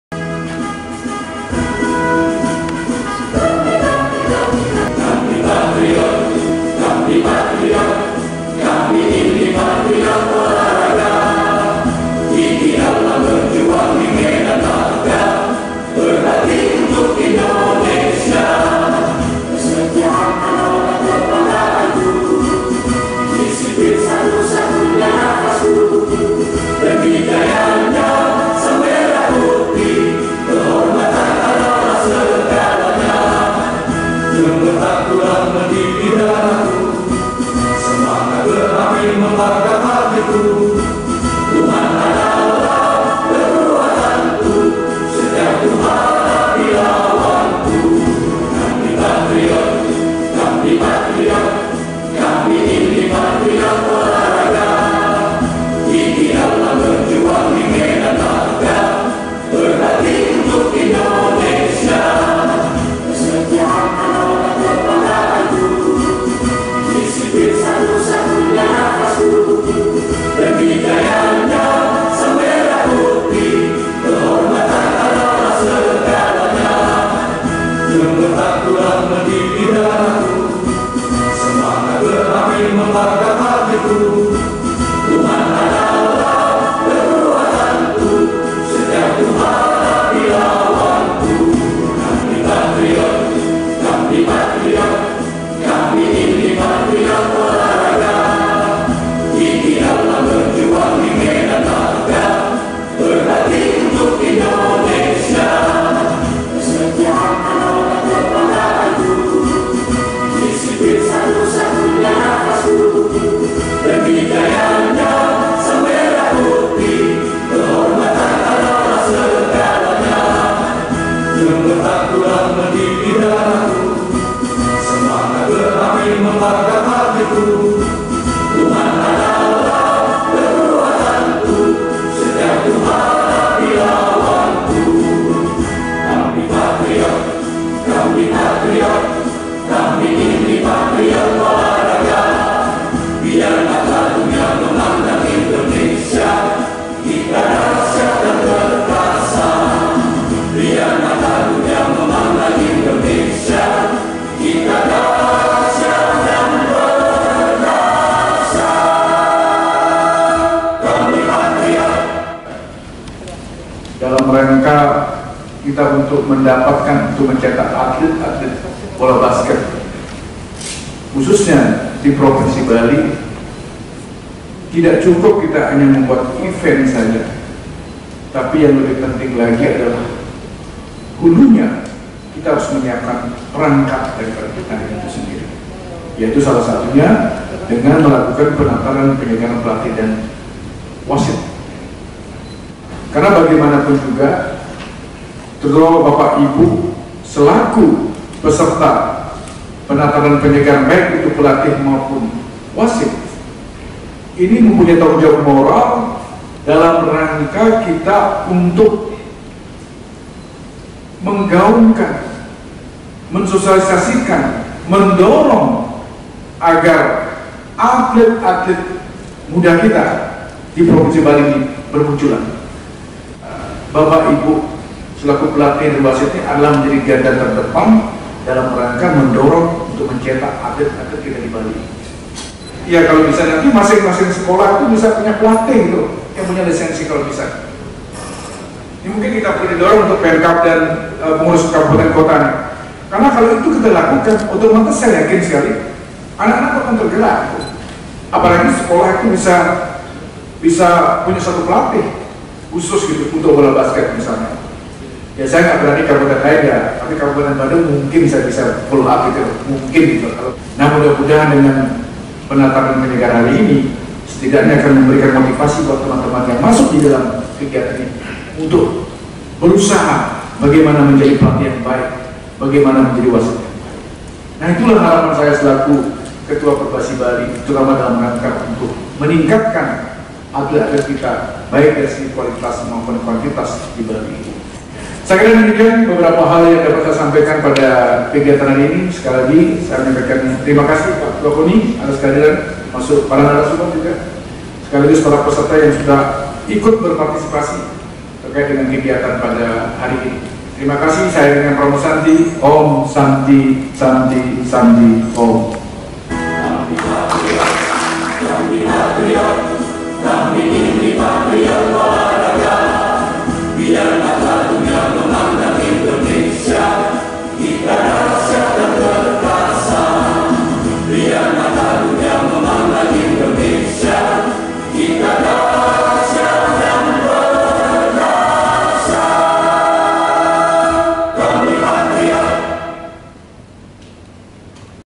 PENGPROV PERBASI BALI GELAR PENATARAN PELATIH & WASIT BOLA BASKET BERLISENSI
PENGPROV PERBASI BALI GELAR PENATARAN DAN PENYEGARAN PELATIH DAN WASIT BOLA BASKET BERLISENSI

BULETIN DEWATA, BADUNG - Penataran dan penyegaran pelatih dan wasit bola basket digelar Pengprov Perbasi Bali di gedung Puspem Badung Mangupraja Mandala, 14 - 24 Juli 2022.

Kegiatan ini bertujuan untuk meningkatkan pembinaan dan prestasi insan bola basket di Bali dengan cara menstandarkan kapasitas pelatih serta wasit, sehingga dapat mencetak bibit - bibit atlet berkualitas, melalui ekosistem perbola basketan di Bali khususnya, dan Indonesia pada umumnya.

Kegiatan ini diikuti sebanyak 77 orang peserta dari Bali, dan dari luar Bali, yaitu dari Jakarta dan Jawa Timur. Peserta meliputi pelatih sebanyak 56 orang, terdiri dari 35 orang yang mencari lisensi C (muda), dan 21 orang yang mencari lisensi B (madya). Sementara peserta wasit berjumlah 21 orang, terdiri dari 9 orang mencari lisensi C, dan 12 orang mencari lisensi B2.

Ketua Umum Perbasi Bali I Wayan Adi Arnawa menerangkan, pelatih dan wasit bola basket dalqm kegiatan ini, memiliki tanggung jawab moral untuk dapat mensosialisasikan, sekaligus mendorong munculnya atlet - atlet muda bola basket di Provinsi Bali.

Melalui pengurus Perbasi ditingkat kabupaten kota se - Bali, akan mengupayakan adanya pelatih basket yang berlisensi di masing - masing sekolah, guna pembinaan olah raga basket.

Mendukung hal itu, Ketua Koni Bali, I Gusti Ngurah Oka Darmawan sangat mendorong Pengprov Perbasi Bali dapat mendata seluruh pelatih dan wasit basket di yang memiliki linsesi saja yang bisa memimpin  event pertandingan bola basket.